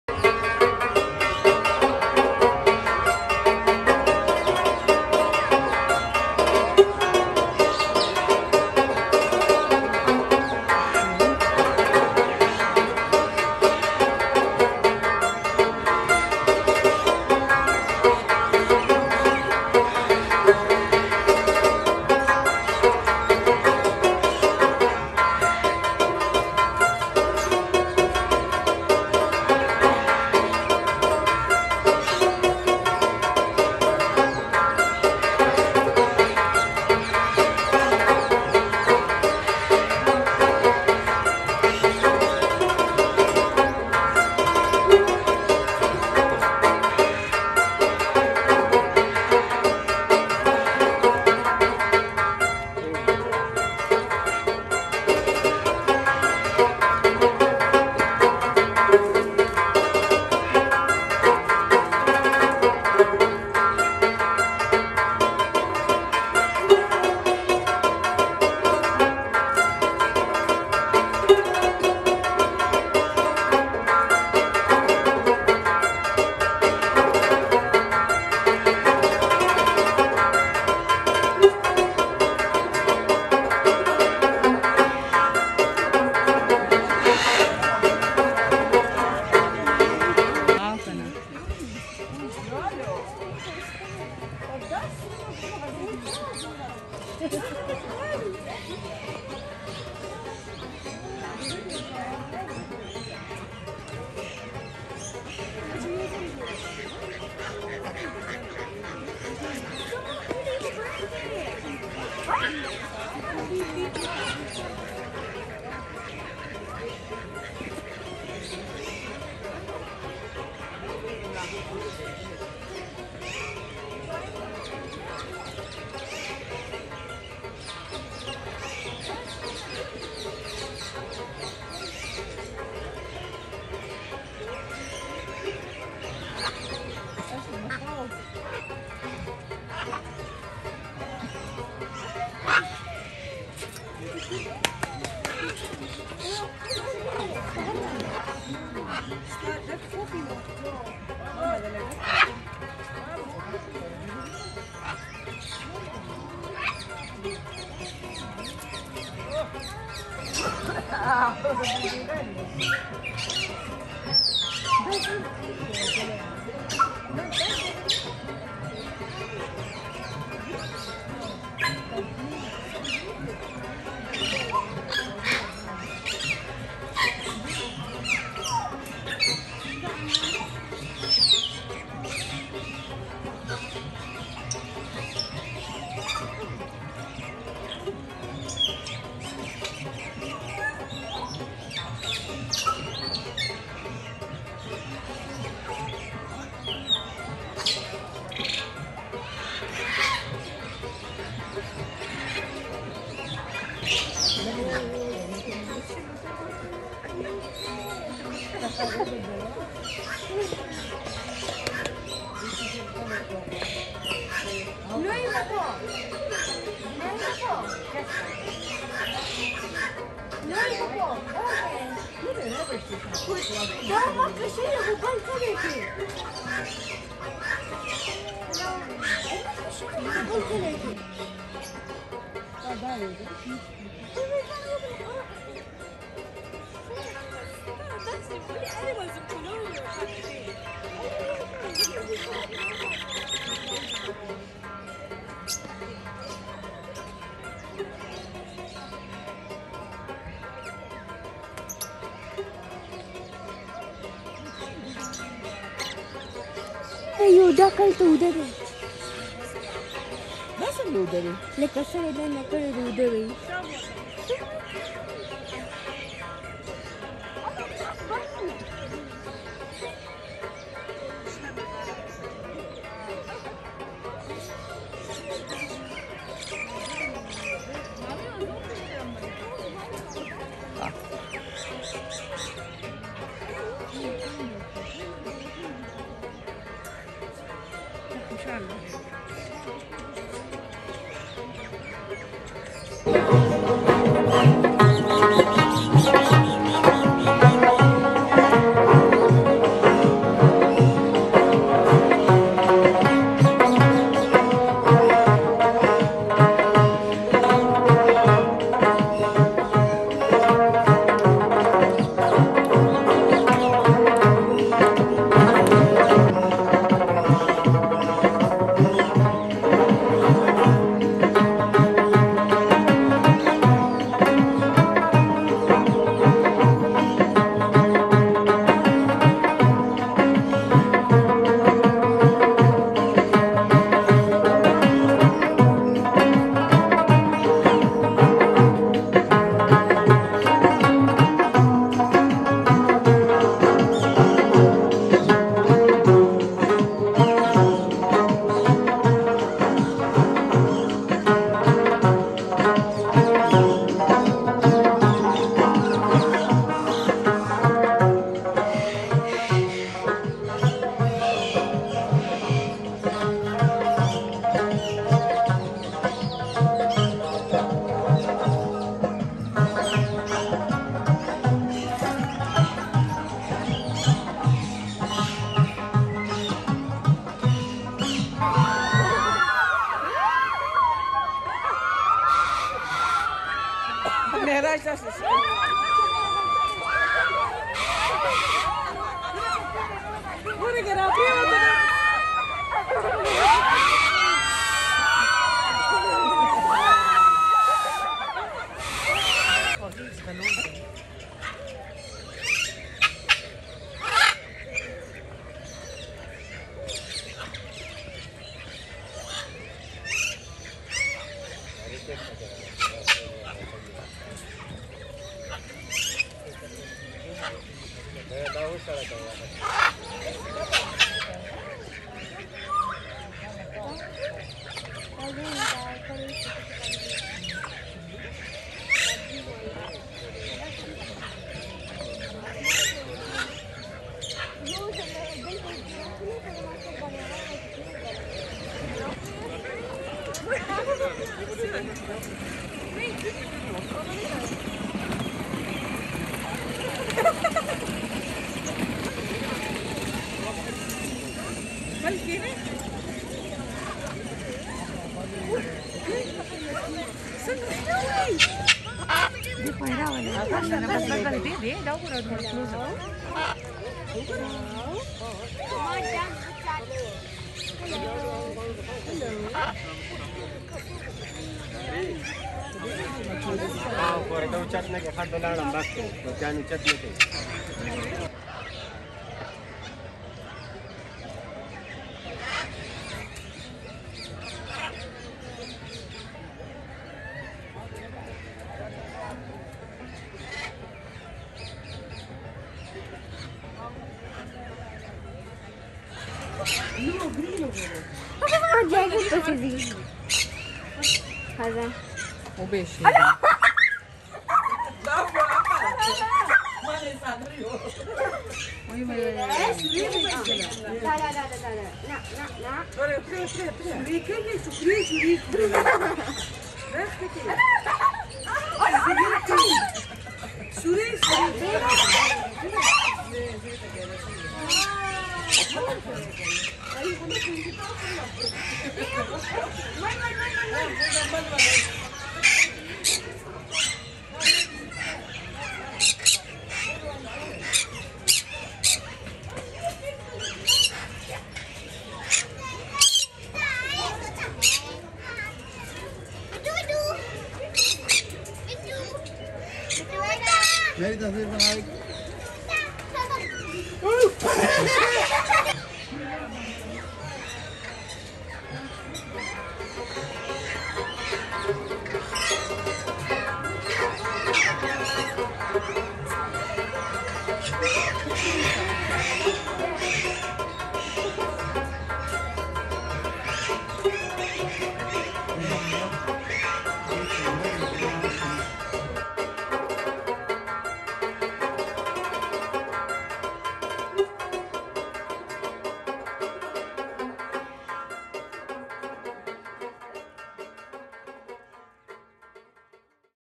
Birds Park Lake View Islamabad
The Bird Park is the worlds' third Largest Free-flight Walk-in Aviary. Be sure to bring your camera. More than 300 Bird species.
Bird Park is situated in Lake view Park Islamabad.